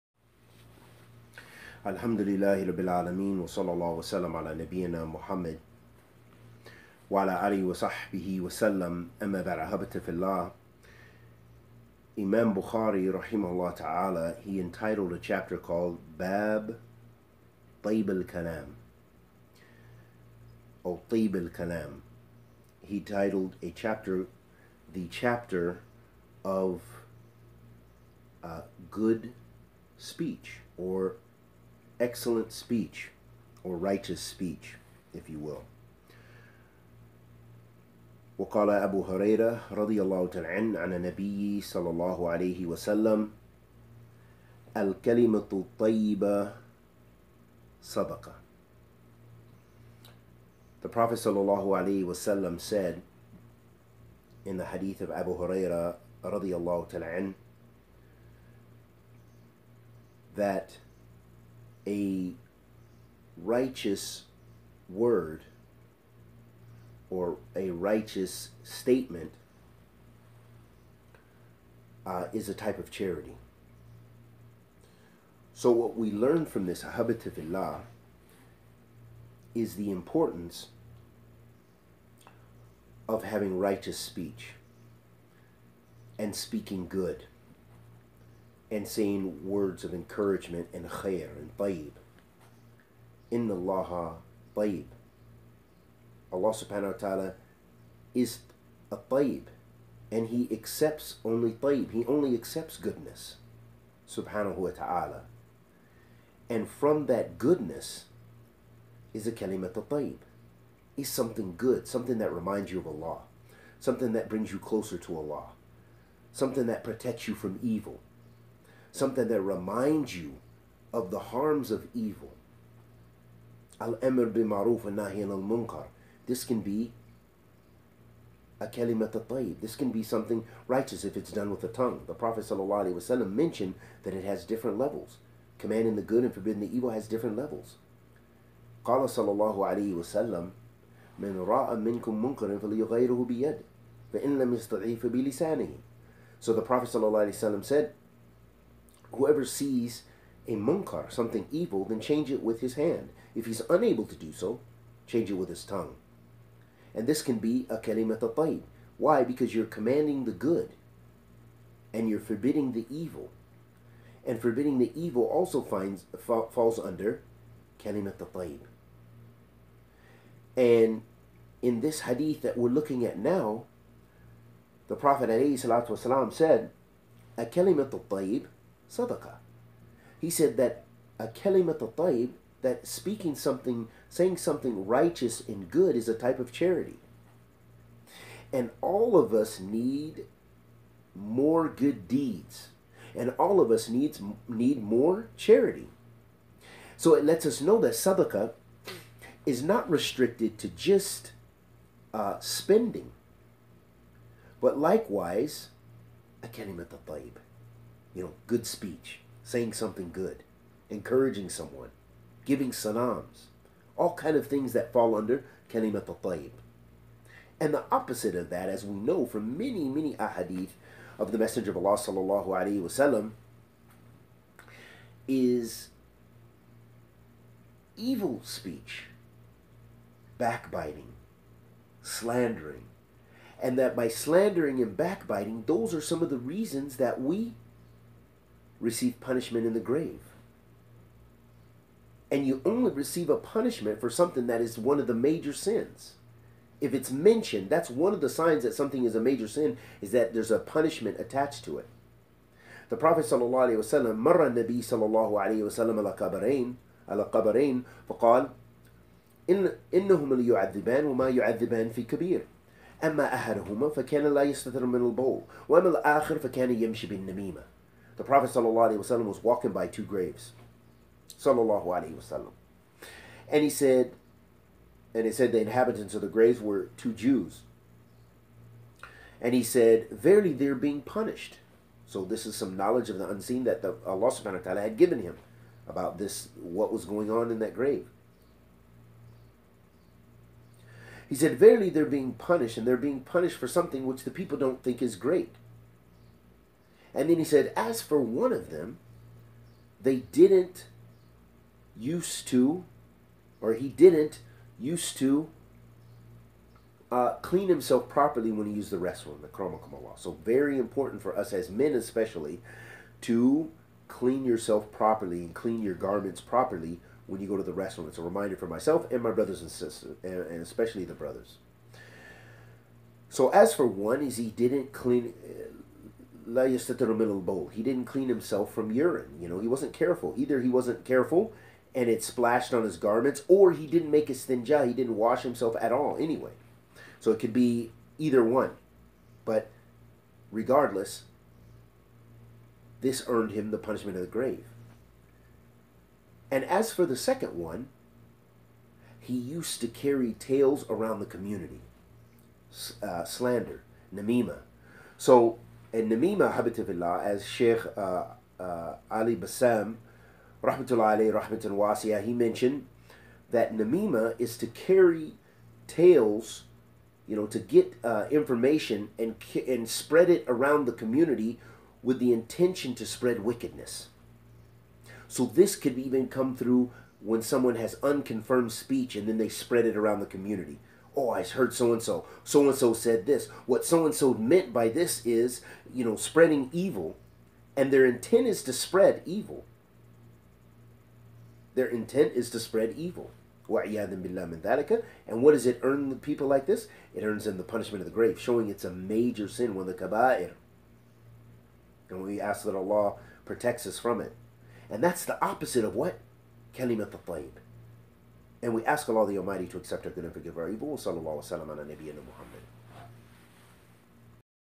Good Speech is Charity: how much do you give? hadeeth
Speaking a good positive word is charity